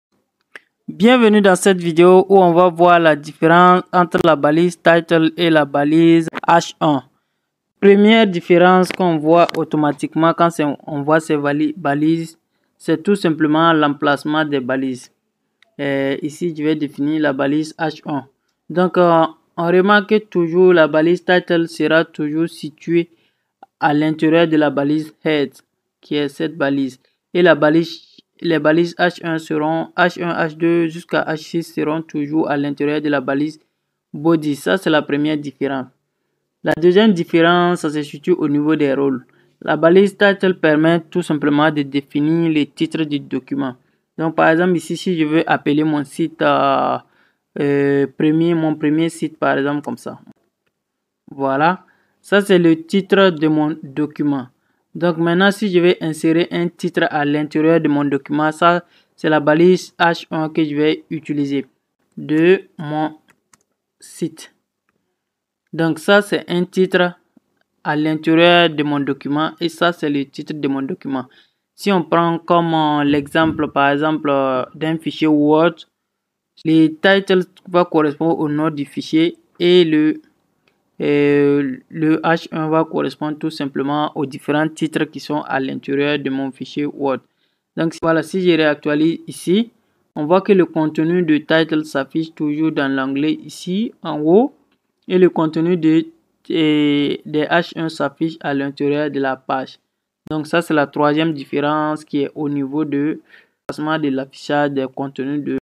Comprendre la Différence entre la Balise Title et H1 en HTML - Tuto Complet
Dans ce tutoriel complet, apprenez la différence cruciale entre la balise title et la balise h1 en HTML. Découvrez comment chacune joue un rôle unique dans la structure de votre page web et son référencement. Parfait pour les débutants et les développeurs web confirmés, ce guide vous aidera à optimiser vos pages pour les moteurs de recherche et à structurer correctement votre contenu.

👍 Likez et partagez si vous avez trouvé cette vidéo utile !
💬 Laissez vos questions en commentaire, je suis là pour vous aider !